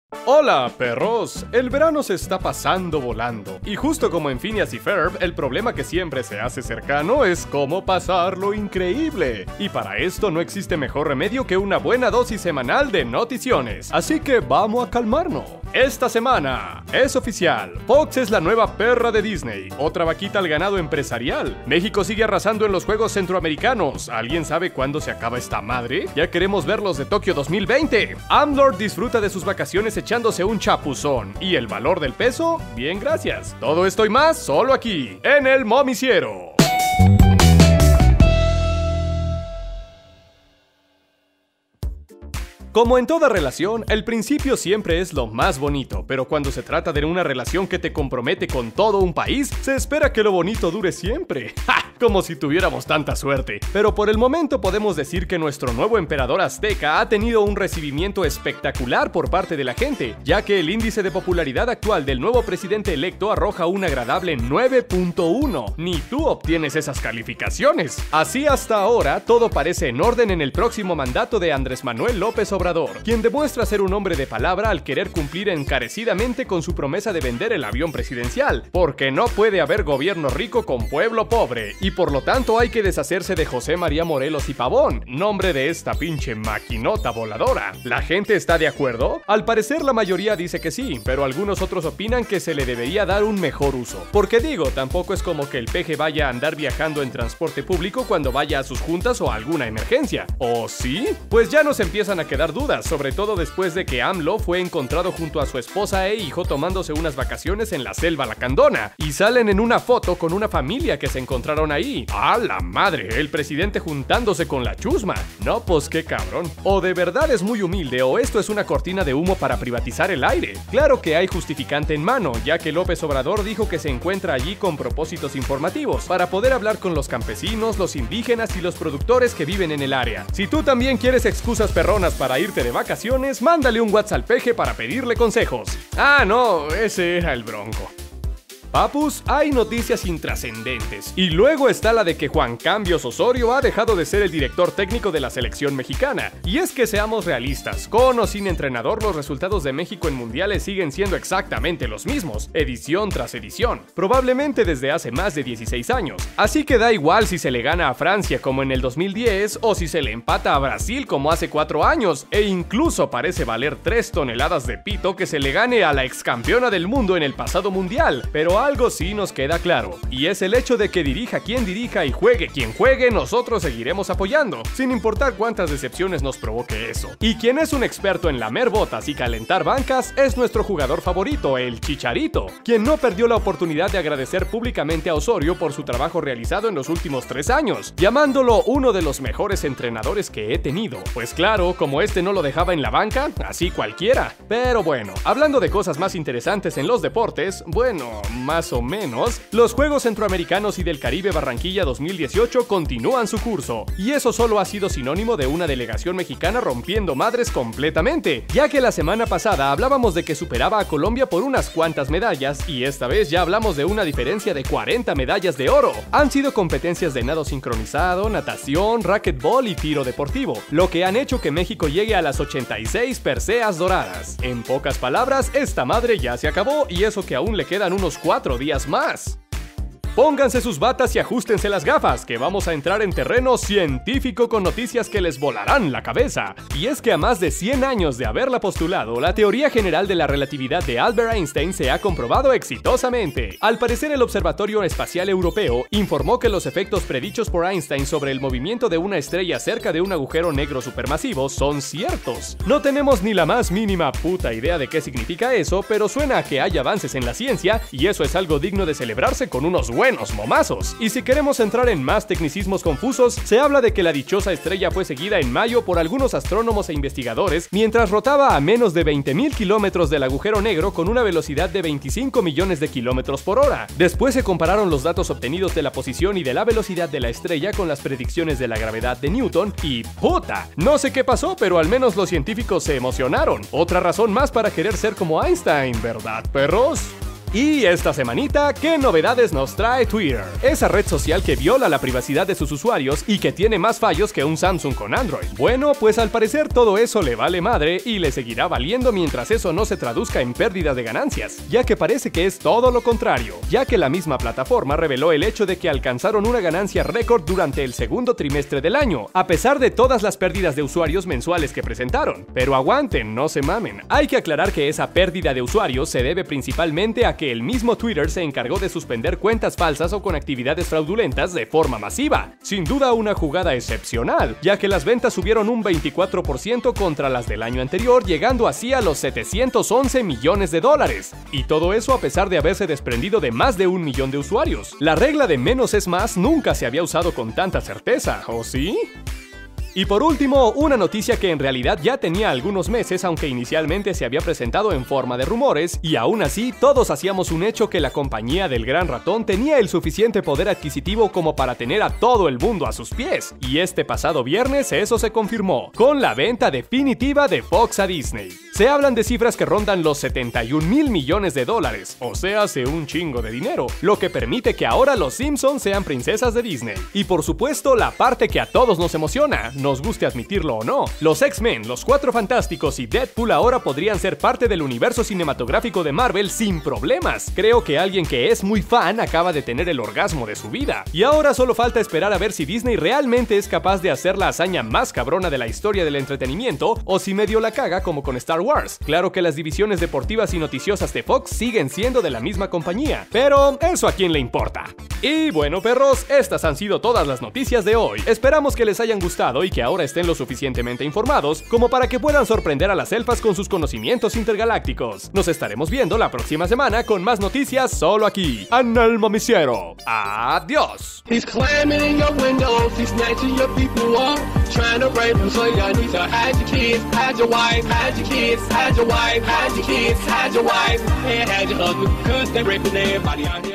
El Momiciero - Disney obtiene la gema de Fox, AMLO vacacional y Más
CRÉDITOS:

-Voz en off: Rodolfo Balderas
-Edición/Musicalización: Josué Alaniz
-Guión: Ludwig Ruano